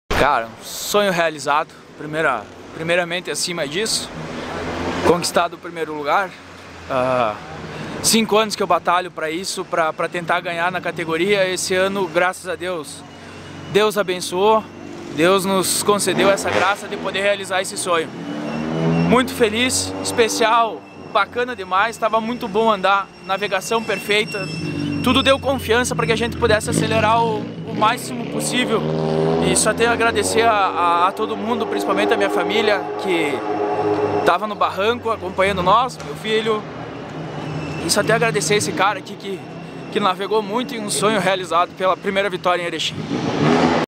Cristiano Beus Final Rally de Erechim 2018
2º Evento do Campeonato Brasileiro de Rally de Velocidade em Erechim (RS) entre os dias 24 e 27 de maio. e nas redes @rallybroficial